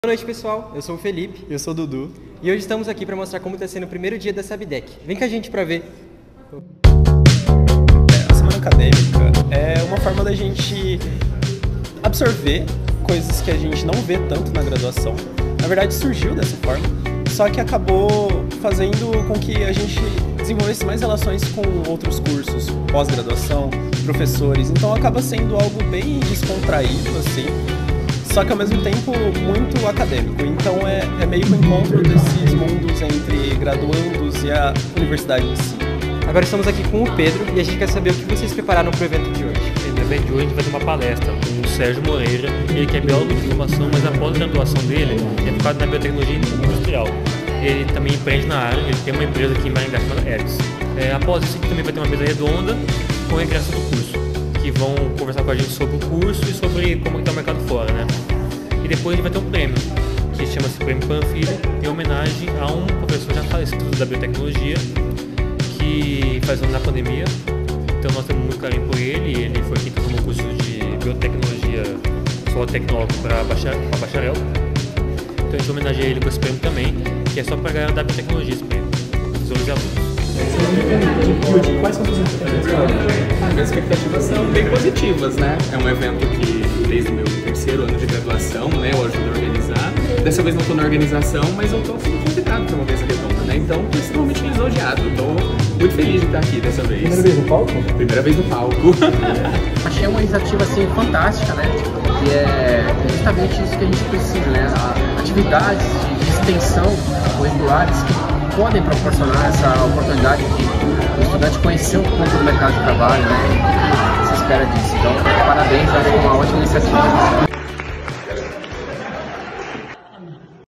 Influem na Sabtec UEM
Saiba mais como foi o evento realizado pelo estudantes de Biotecnologia pelo quadro Correspondentes do Influem!